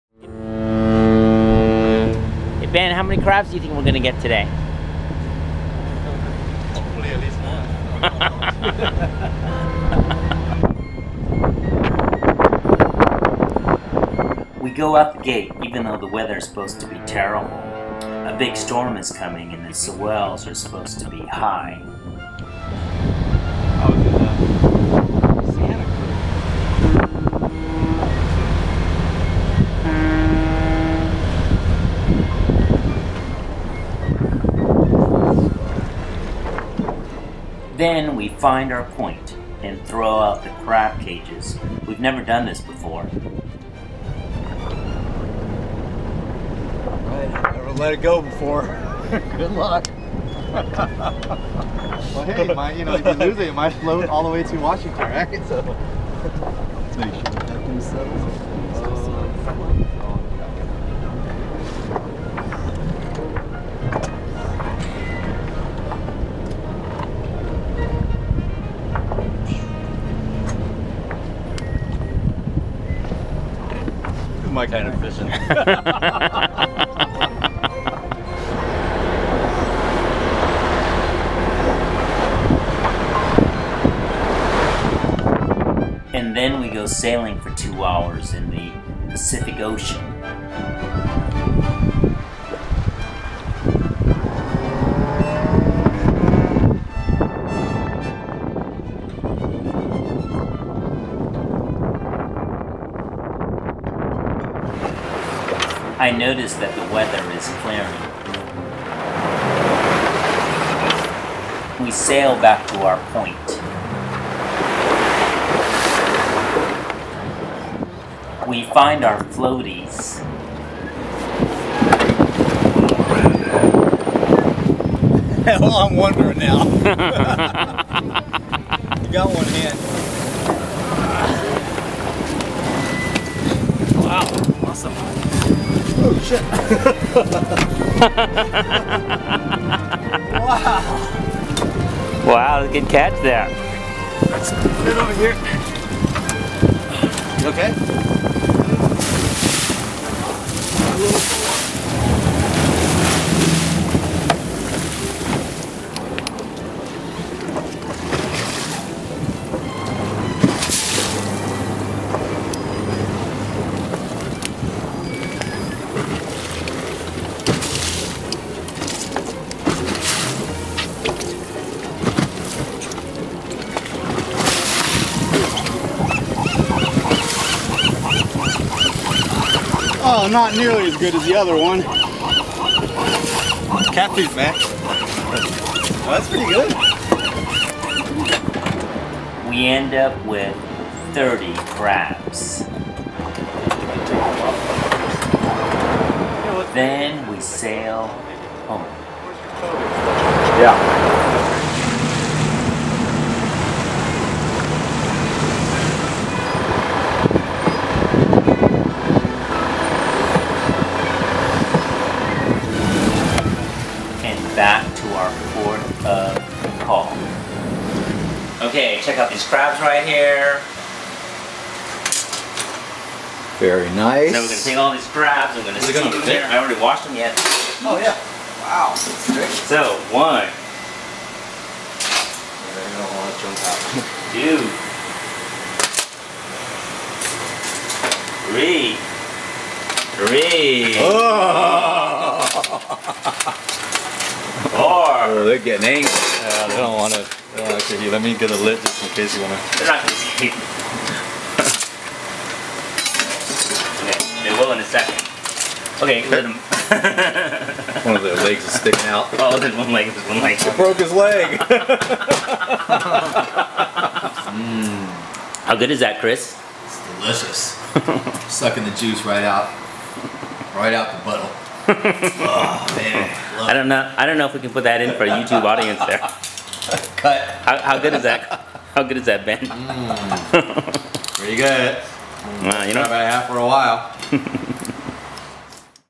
Our Crab Sail
We go Crab Sailing. I've never done this before.